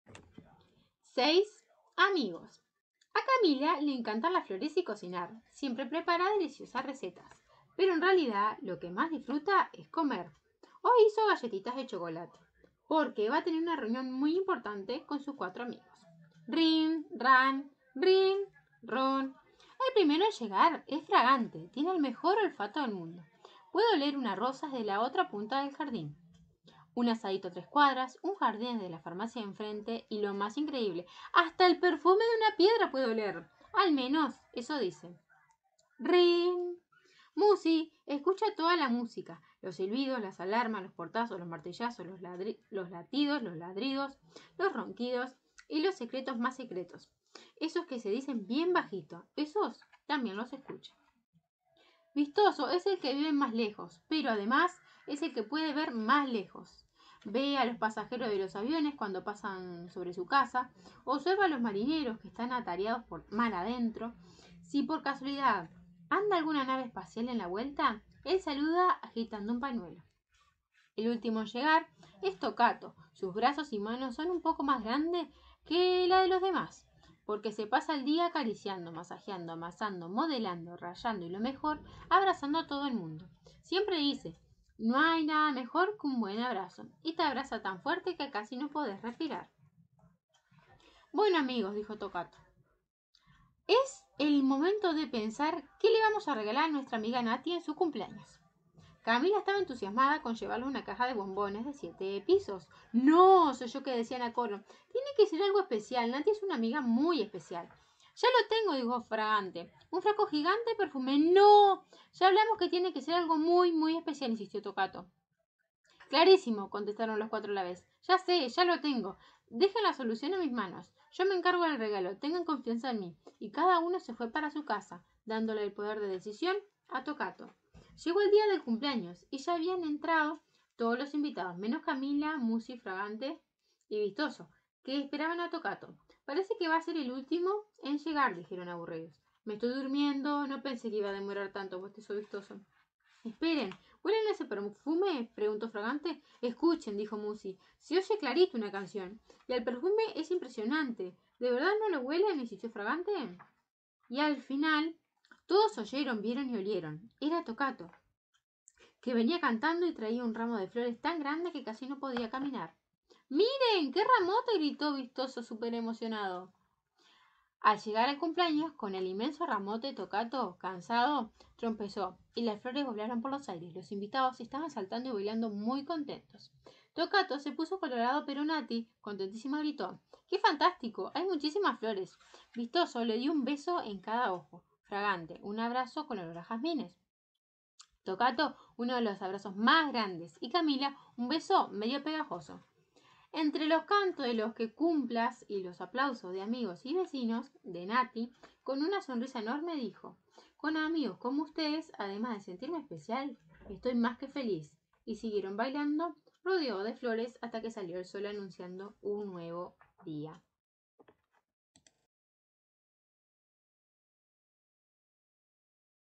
Seis amigos de Susana Olaondo
Autor: Susana Olaondo
Sinopsis: Comprar un regalo de cumpleaños es difícil cuando se trata de una amiga muy especial. Y mucho más cuando los amigos que tienen que elegirlo no son nada comunes.